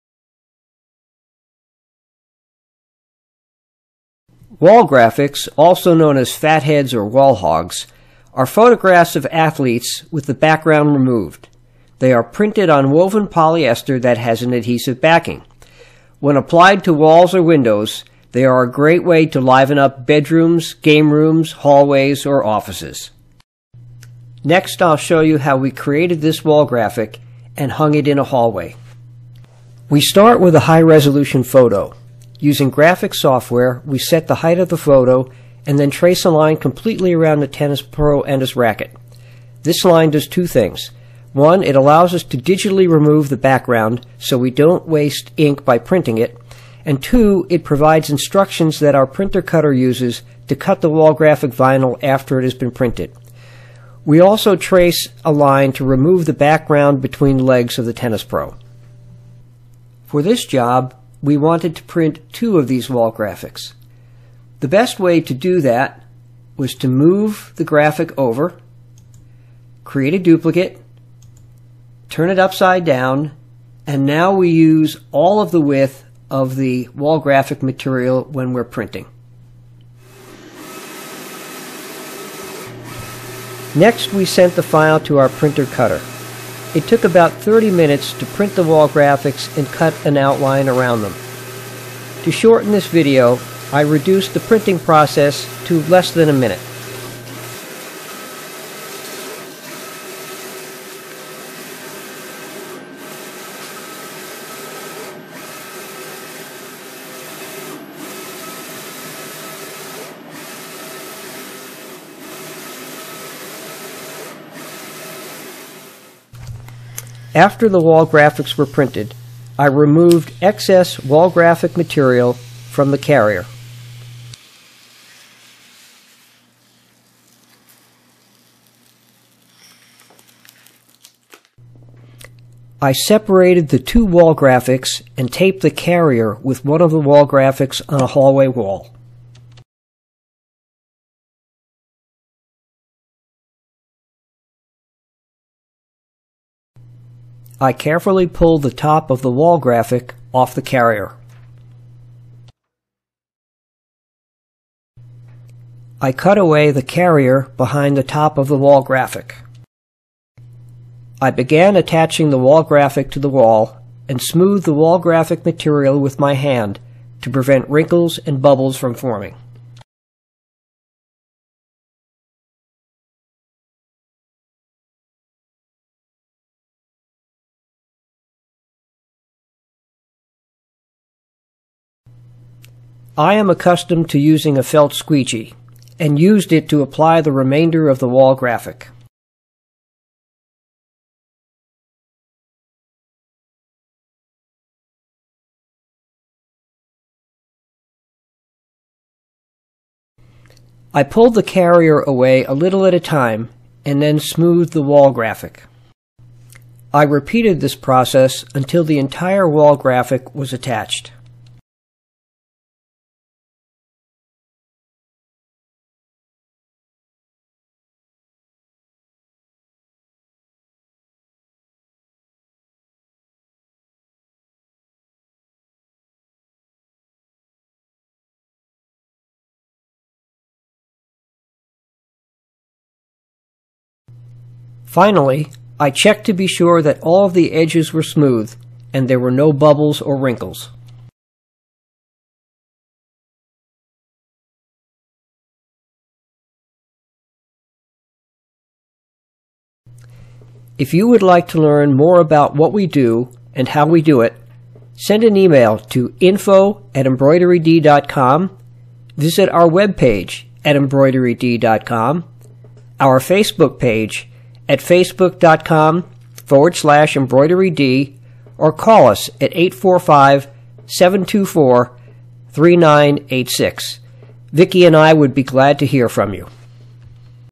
Creating and hanging a wall graphic
This video shows how Embroidery Designs created and hung a life-size wall graphic.